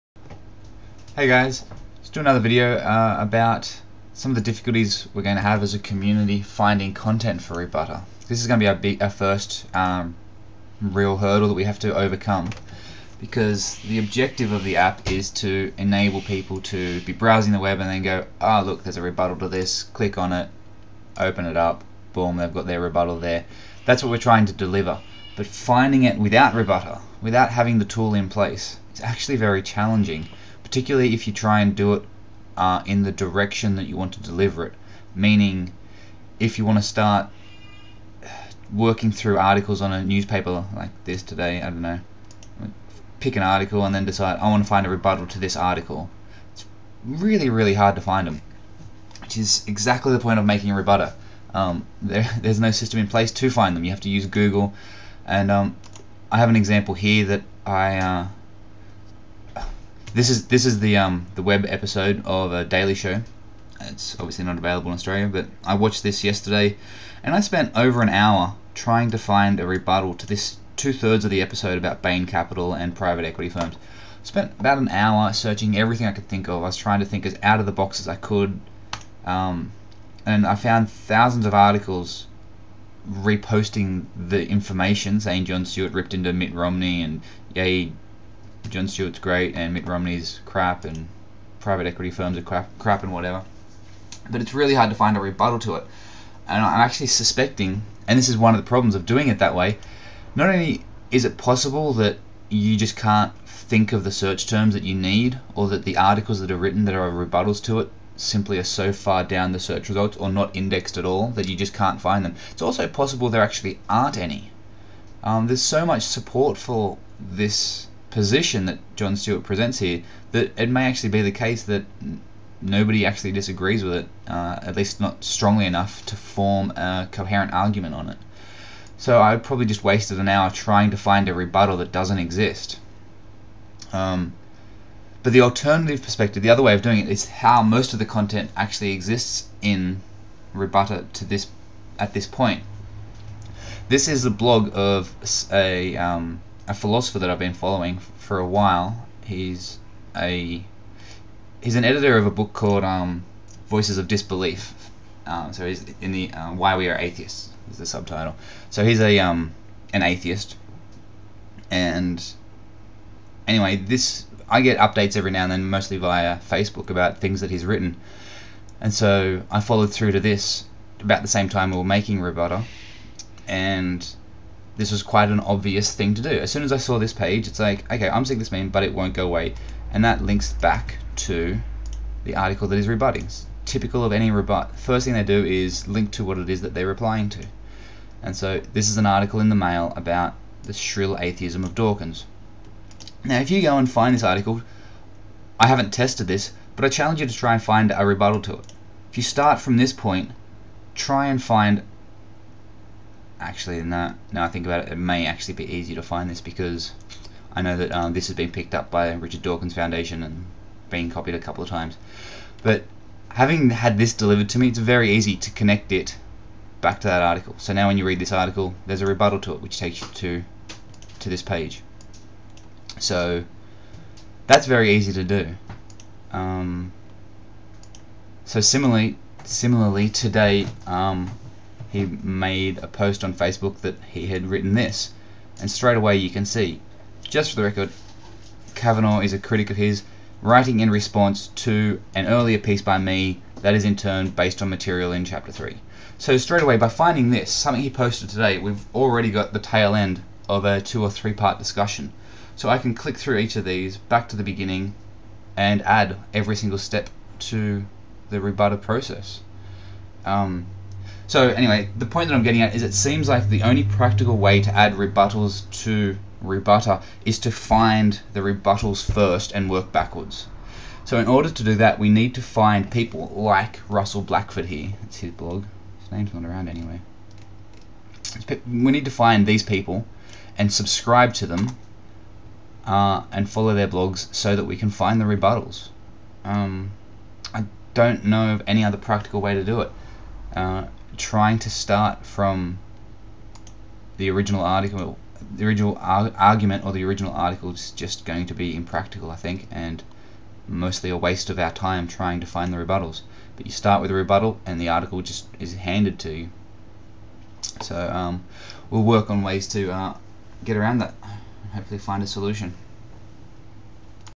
rbutr - Adding rebuttals to rbutr
It turns out that finding rebuttals to articles can be very difficult, and sometimes impossible. In this way, whenever they make a rebuttal, it is simple to add that rebuttal to their original article.

Basically, we need to work 'backwards' in order to allow other users to use rbutr 'forwards'